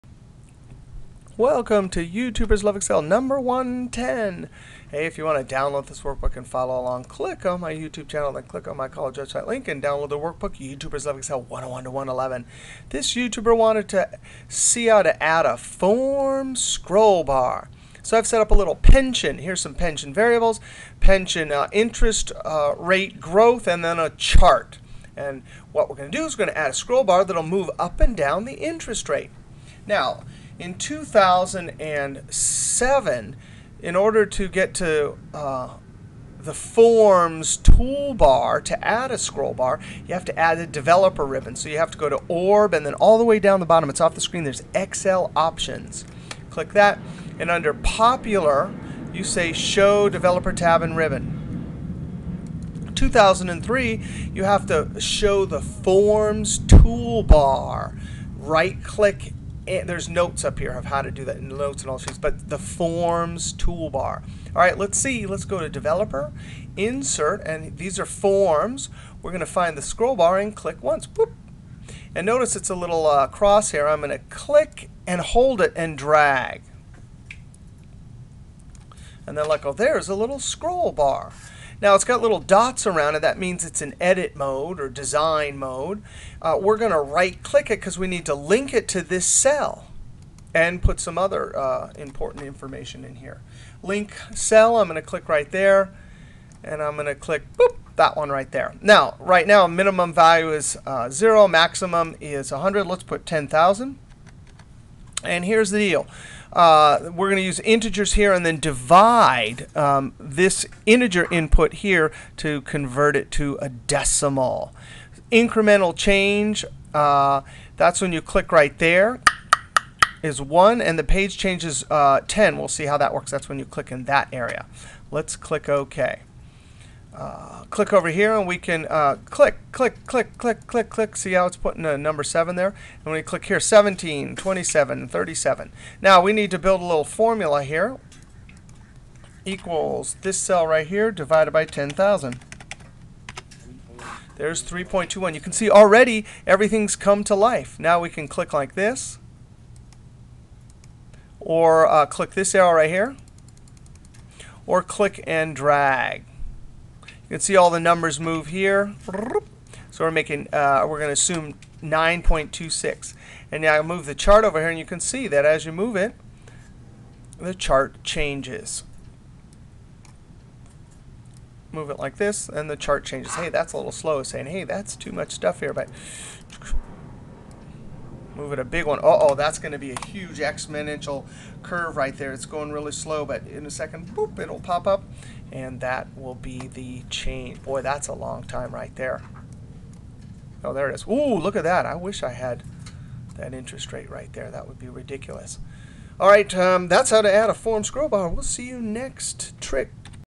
YTLE#110: Excel Forms Scroll Bar Linked To Cell
In Excel, see how to add a Forms Scroll Bar Linked To a Cell.